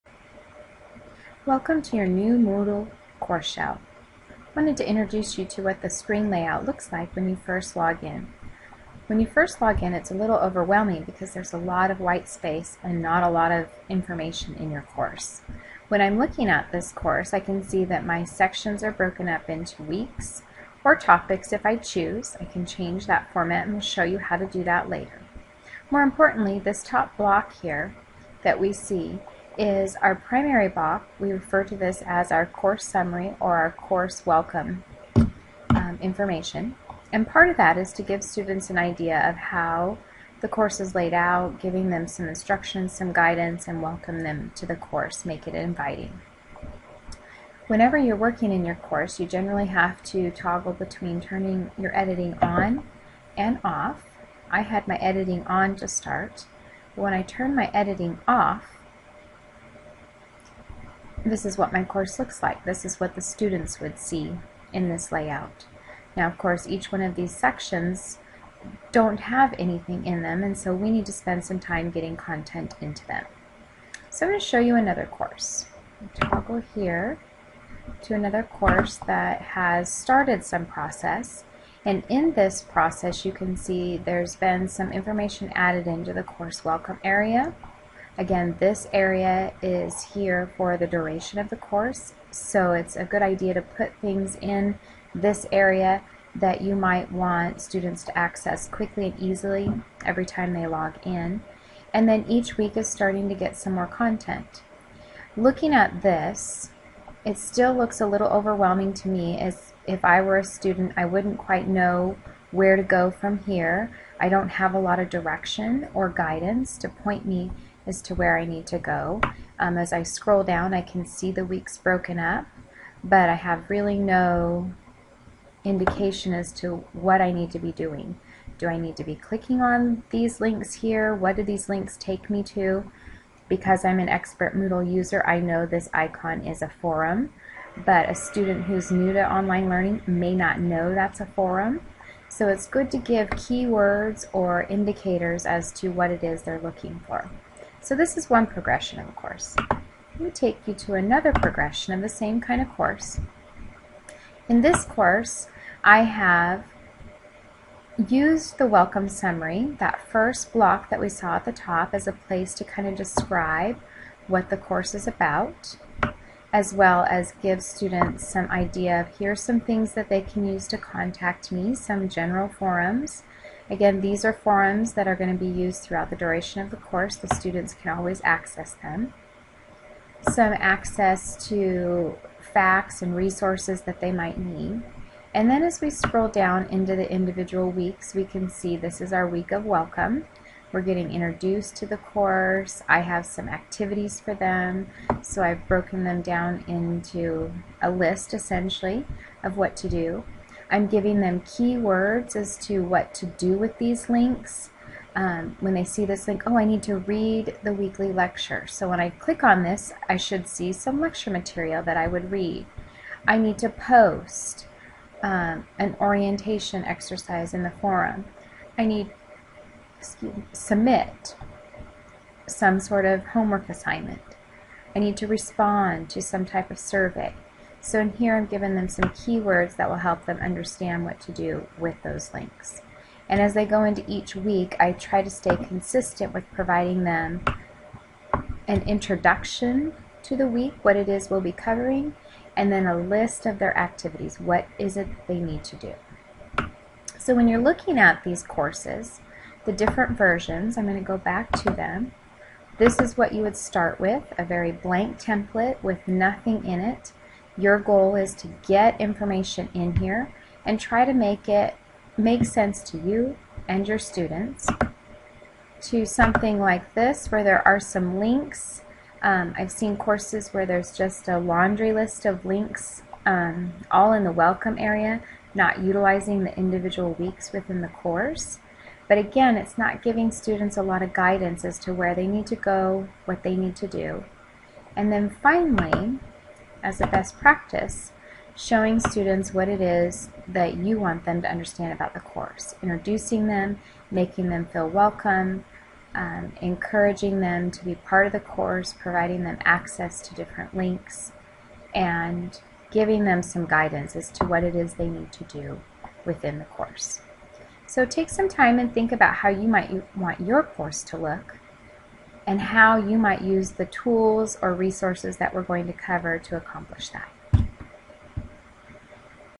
Working with your new Course Shell
This video shows you the many progressions of working with a cours shell.